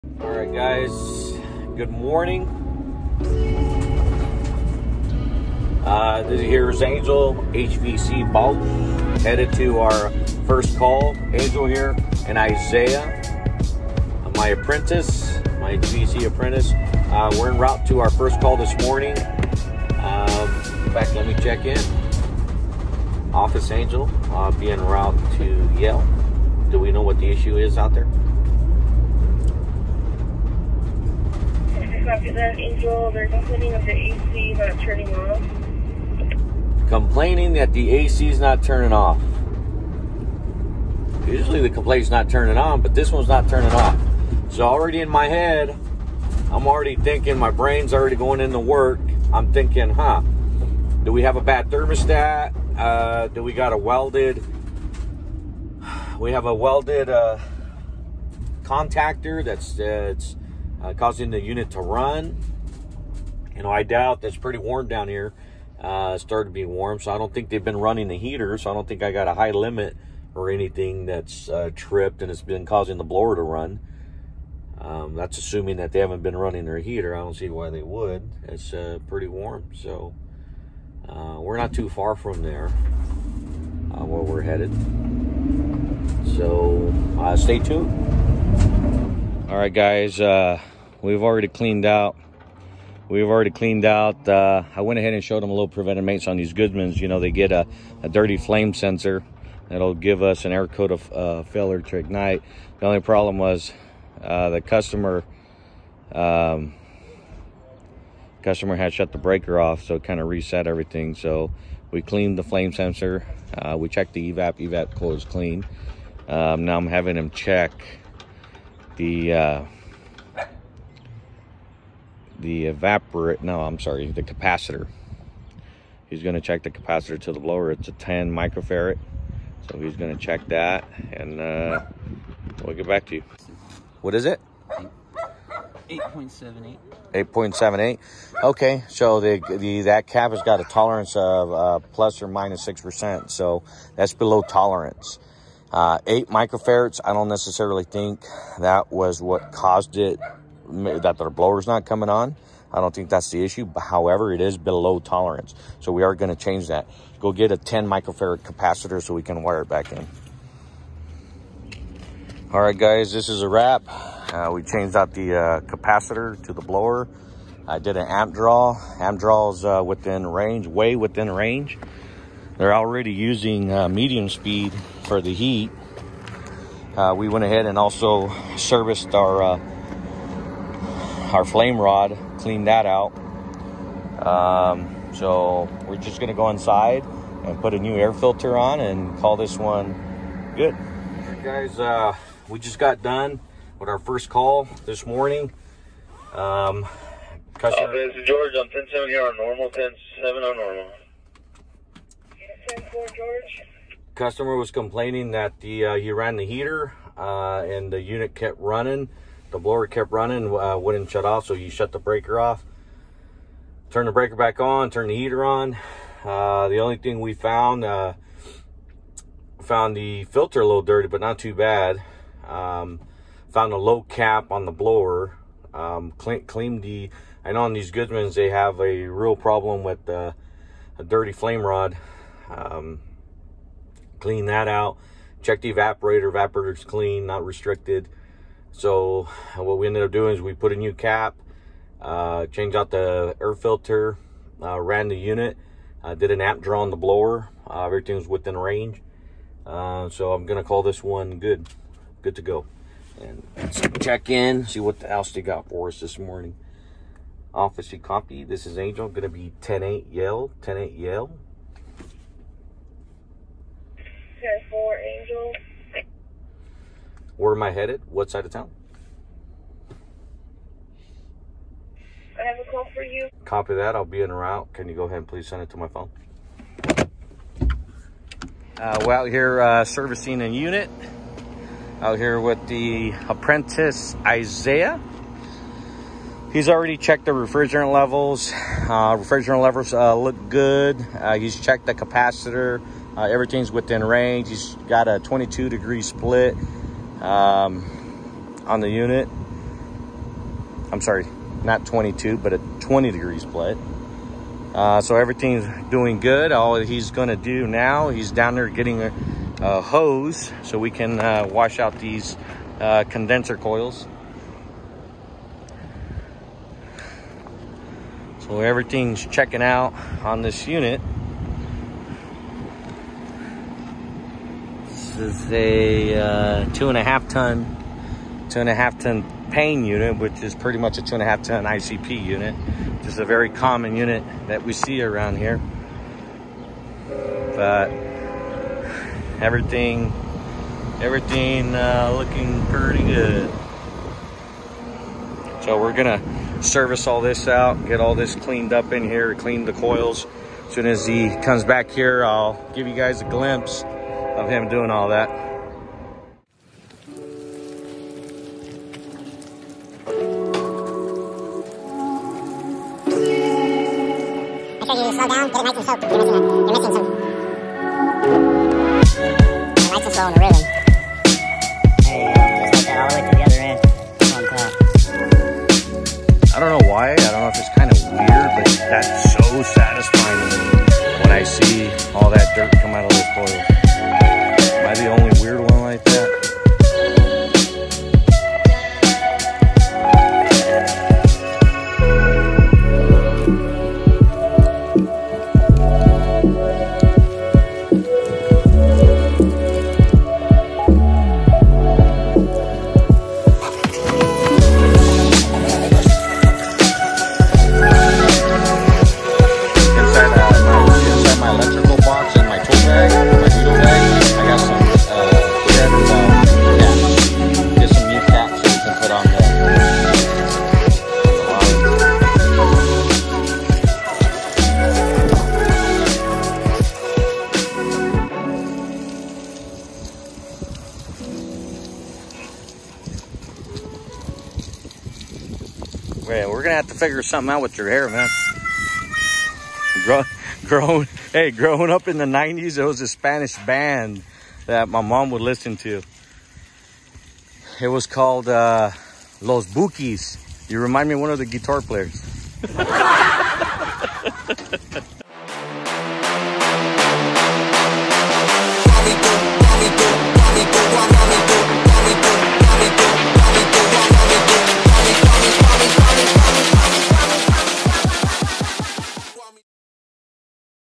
HVAC TECH IN TRAINING
Today I will be taking my HVAC apprentice for a ride along to show him how to diagnose and service an HVAC system.

More Content On The Way Soon!